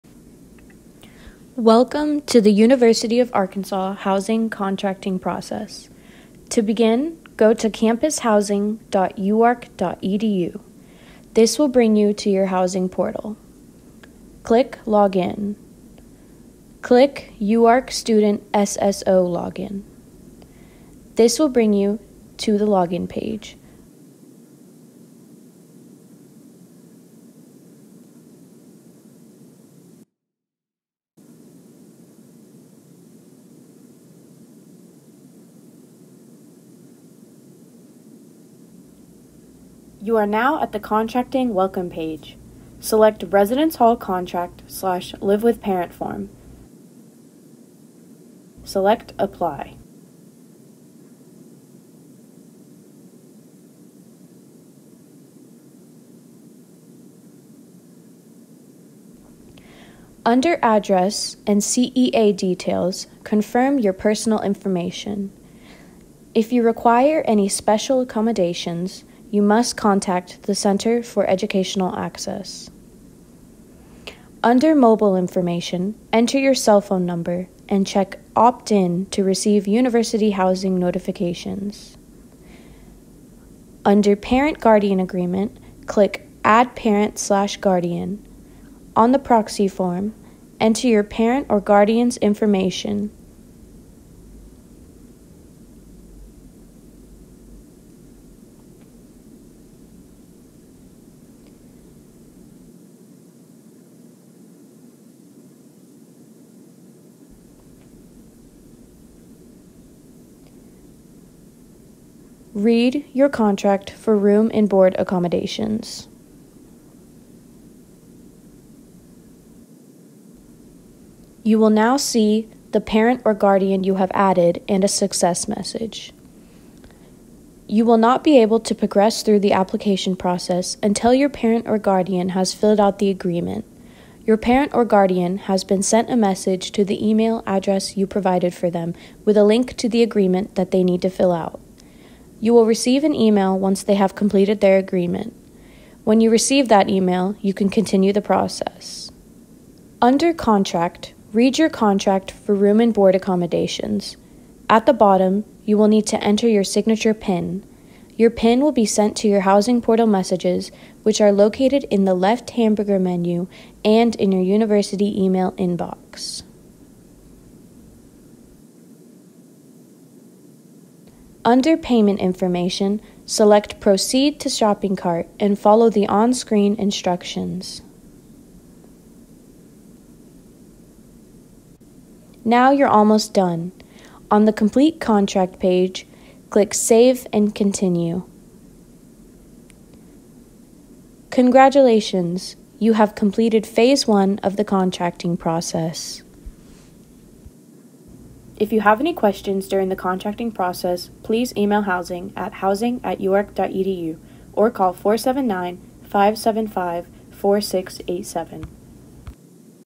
University Of Arkansas Housing Contract - Phase 1
This video describes how to complete Phase 1 of the University of Arkansas Housing contract.

to start the contracting process